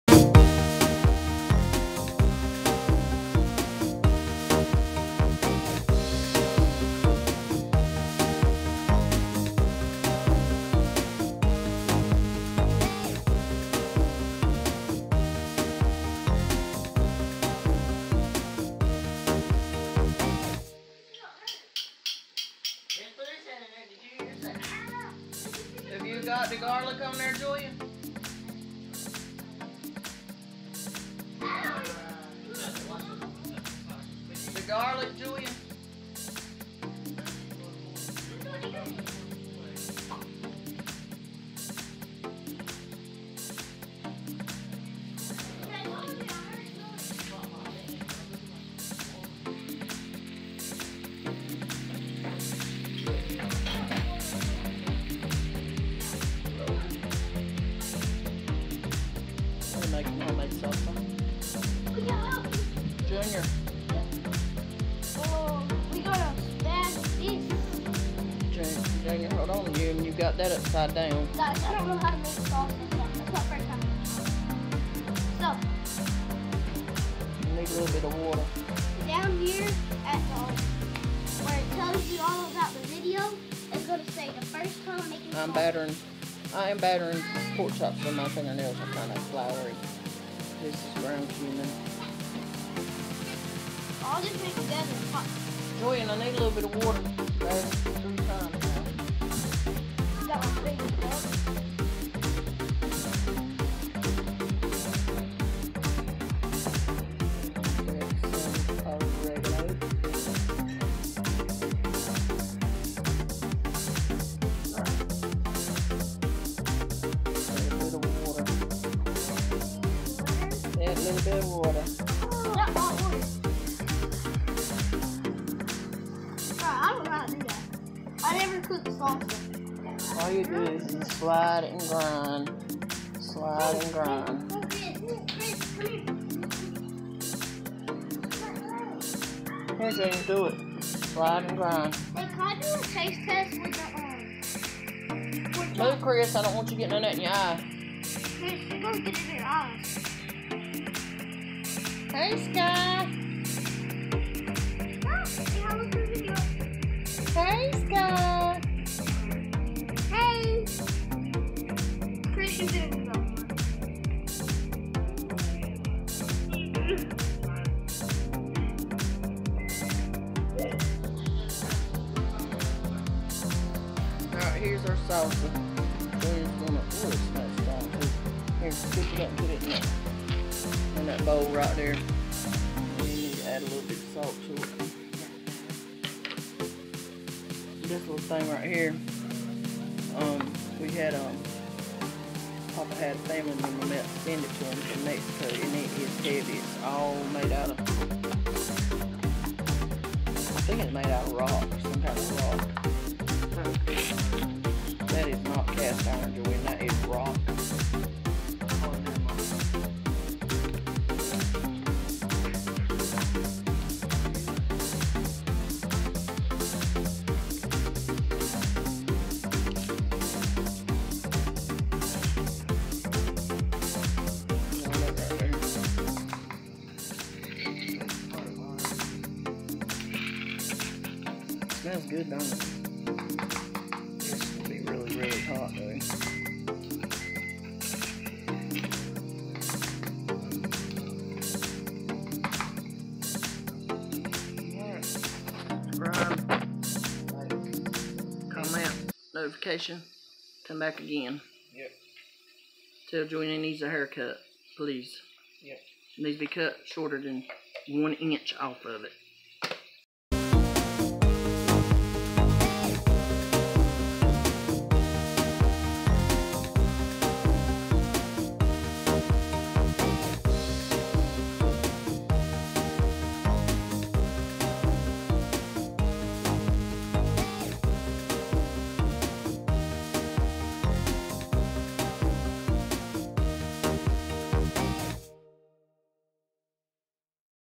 Making Salsa with Jalapenos in Stone Mortar and Pestle Molcajete
So we made Salsa in our Stone Mortar and Pestle Molcajete with Jalapenos , Tomatoes , Cumin ,  Oregano  and Garlic it was so good..
Please check out these VERY active content creators THAT ARE PART OF MY CHANNEL FAMILY .